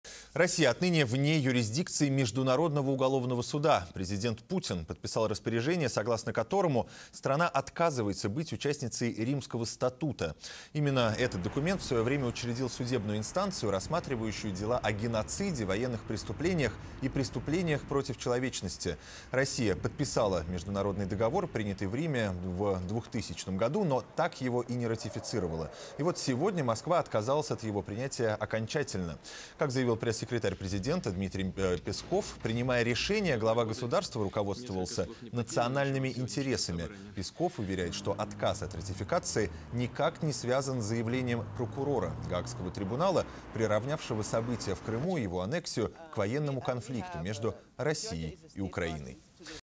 Путин подписал распоряжение об отказе от Римского статута
Россия отныне  — вне юрисдикции Международного уголовного суда. Президент Путин подписал распоряжение, согласно которому страна отказывается быть участницей Римского статута. Именно этот документ в свое время учредил судебную инстанцию, рассматривающую дела о геноциде, военных преступлениях и преступления против человечности. Россия подписала международный договор, принятый в Риме, в 2000 году, но так его и не ратифицировала. И вот сегодня Москва отказалась от его принятия окончательно.